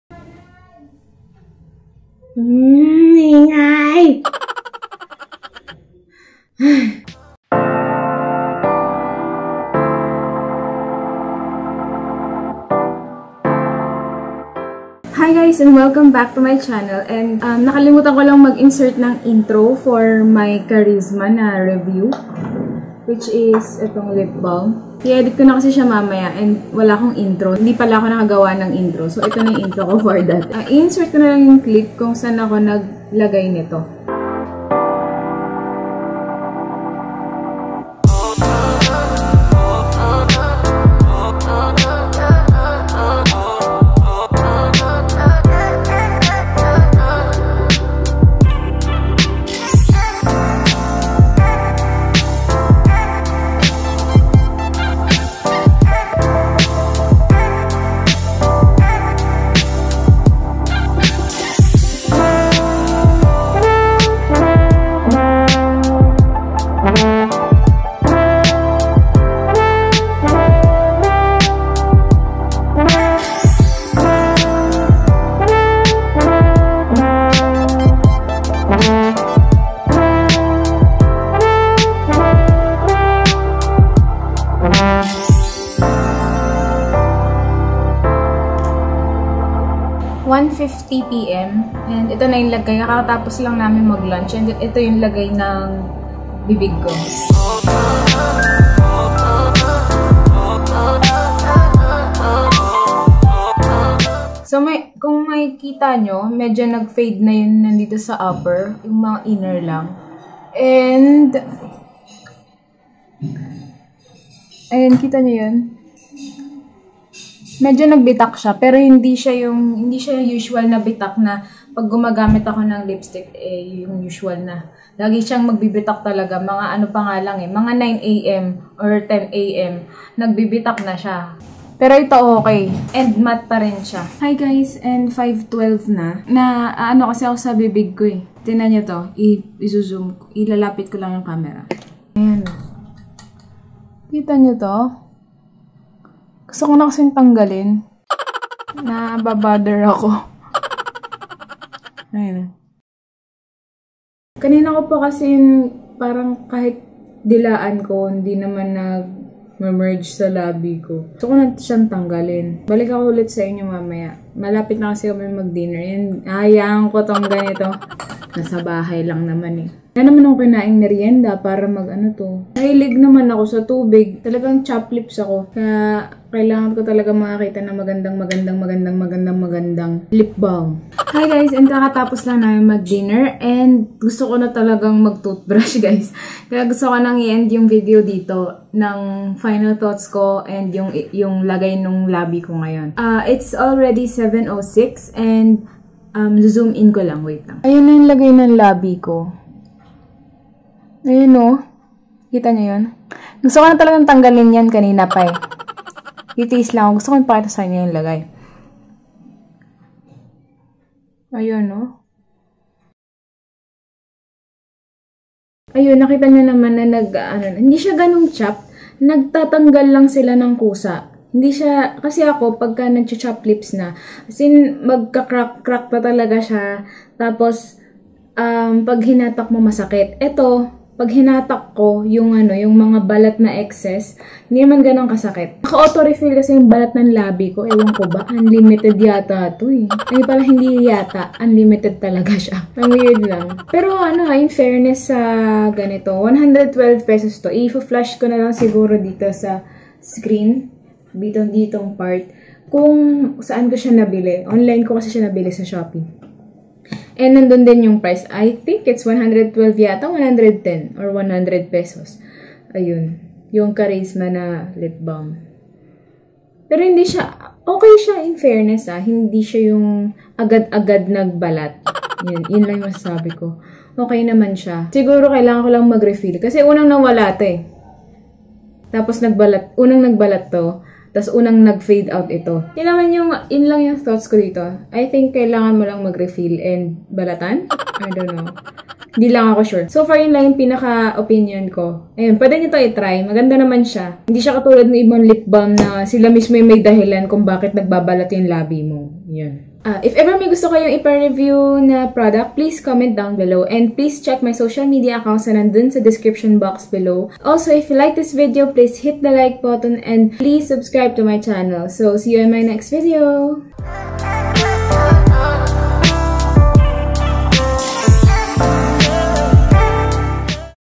Carizma Matte Tinted Lip balm | Maganda ba?
I hope naka tulong ako sa inyo through this video.
BTW. Matte talaga siya. and so sorry nakalimutan ko i-swatch.